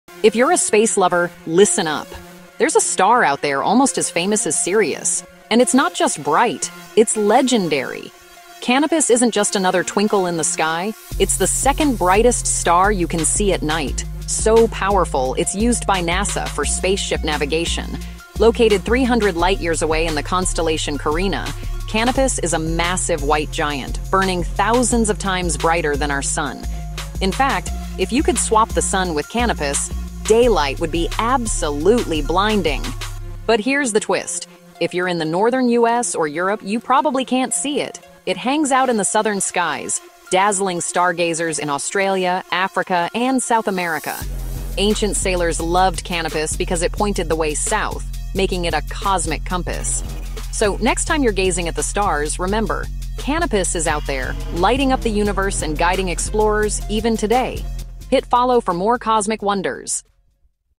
Canopus: The Brightest Star You Never Knew!
Discover Canopus, the brightest star in our night sky that you probably never knew about! 🌟 In this fast-paced YouTube Short, we dive into the captivating facts about Canopus, from its stunning brightness to its unique position in the southern sky. Learn why this stellar giant has fascinated astronomers for centuries and how it stands out among the stars.

Join us on this cosmic journey and get ready to see the universe from a whole new perspective!

OUTLINE:

00:00:00 Canopus: The Brightest Star You Never Knew!